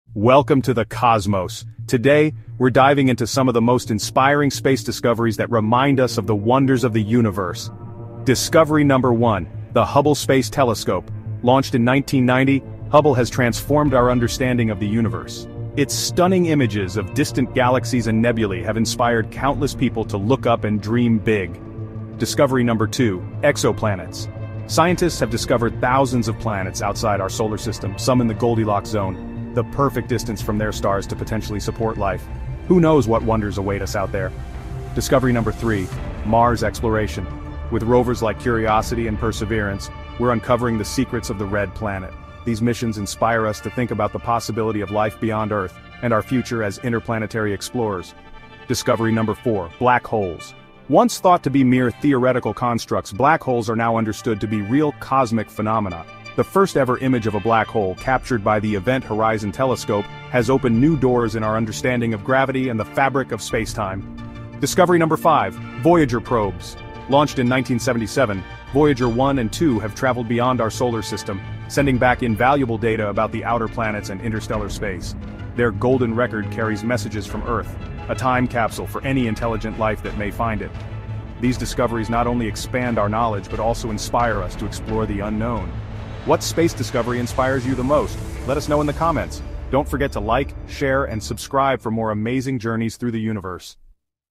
Space Discoveries That Inspire: Exploring the Stars! 🌌
Join us on a journey through the cosmos as we explore inspiring space discoveries! From the breathtaking images of the Hubble Space Telescope to the mysteries of exoplanets and the groundbreaking Mars missions, these discoveries ignite our curiosity and imagination. 🌠 What inspires you about the universe?